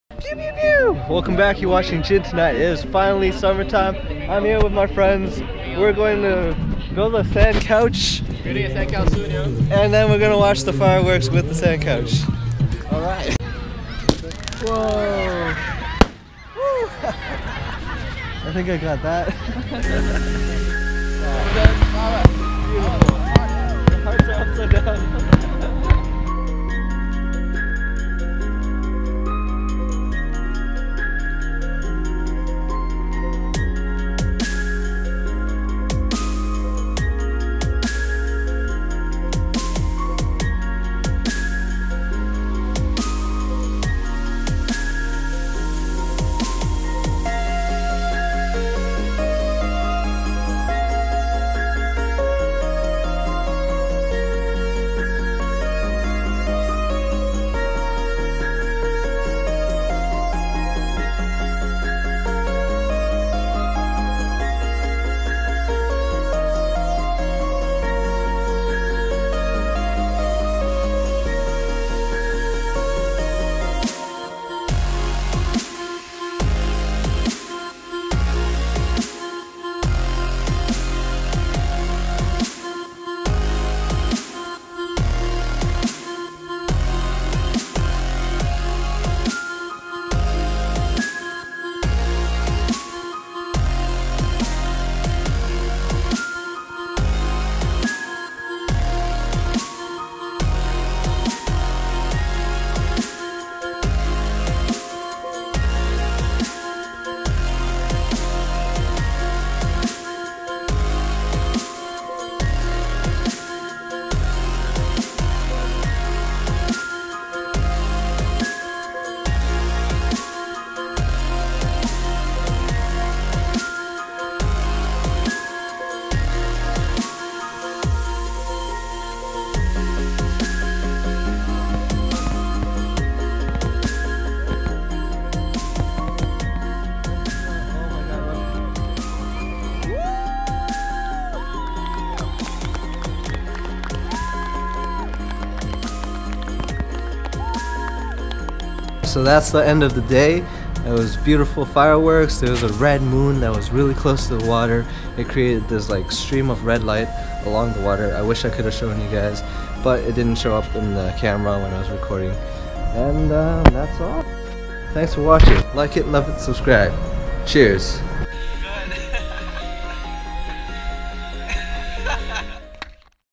Victoria Day Beach Adventure!
Better late than never! Spent the afternoon at the beach and watched the amazing fireworks shower from above